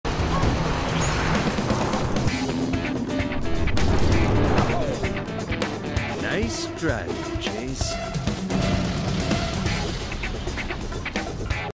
Nice Driving. Chase.
🚗💨 Buckle up and hold onto your hats because on Alt F10 Moments, we're about to redefine the meaning of "Nice Driving" in the most chaotic chase scenes you've ever witnessed! 🏎️🤣

🚓 Get ready for high-speed hilarity as we dive headfirst into the world of "Nice Driving. Chase." Think you've seen crazy car chases before? Think again! Our fearless drivers take the wheel and turn the streets into their personal playground, leaving chaos and confusion in their wake. 🌪️💥

🚨 From narrowly avoiding traffic collisions to accidentally launching off ramps like they're in a Fast and Furious movie, our drivers are experts at turning every chase into a comedy of errors. Will they catch the bad guys, or will they end up causing more mayhem than the criminals themselves? Tune in to find out! 🤪🚓

⚠️ WARNING: Side effects may include uncontrollable laughter, an overwhelming urge to rewatch every action movie ever made, and a newfound appreciation for the art of vehicular chaos. Let's hit the road and see if our drivers have what it takes to chase down the bad guys or if they end up chasing their own tails! 🌟🏁